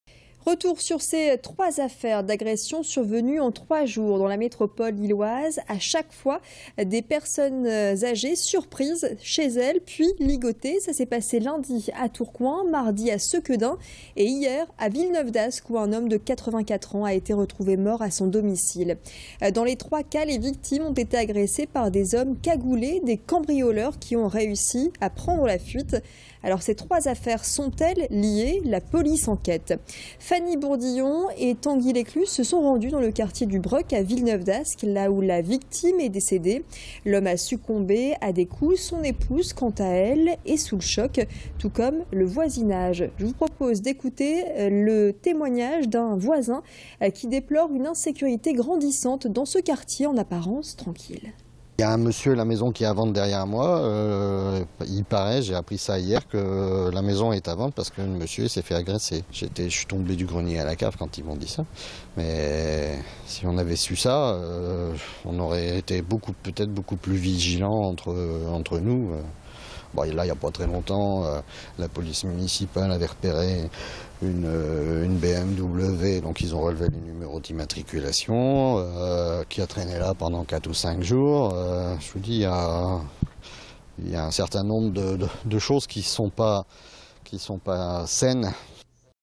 Mort d'un octogénaire lors d'un cambriolage
Trois affaires d'agression survenues en 3 jours présentent d'étranges coïncidences: des personnes âgées sont surprises chez elles puis ligotées. La police enquête.

Sites webs/blogs, copiez cette vidéo avec le bouton "Intégrer" !